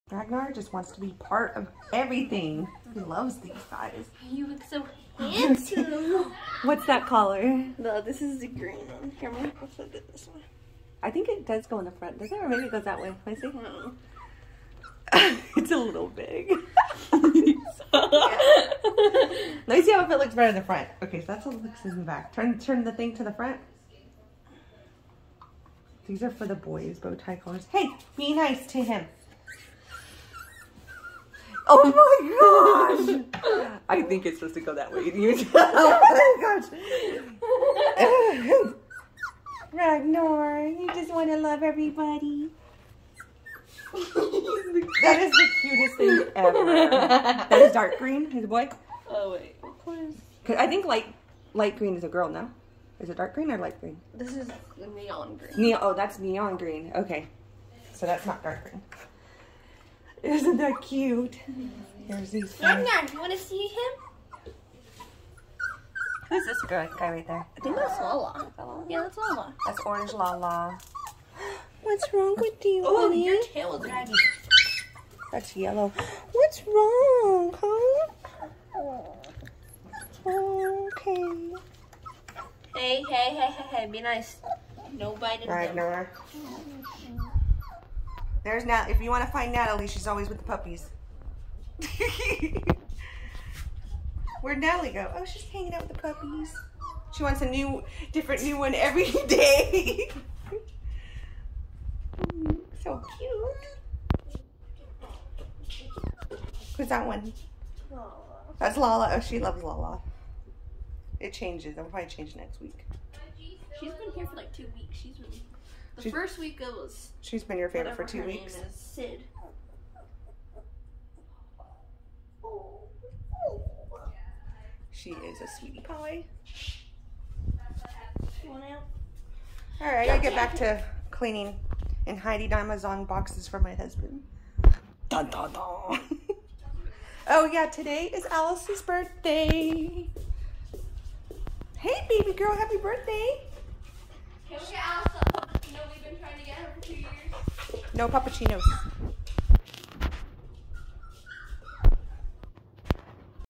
Puppy bow ties came in and I am loving it
F1b goldendoodle puppies. So perfect. 3 weeks old.